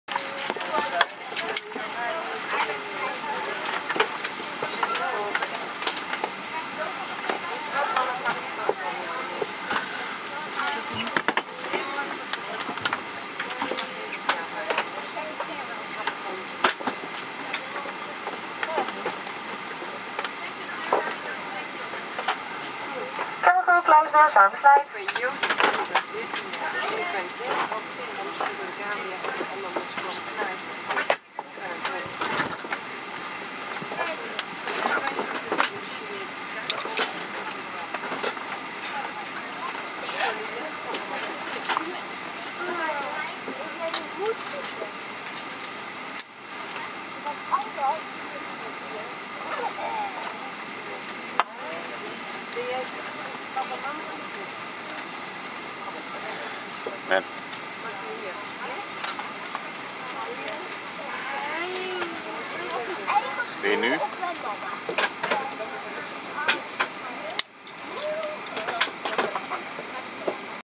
Easyjet Airbus A320 landing at Faro airport
filmed this one while waiting for take off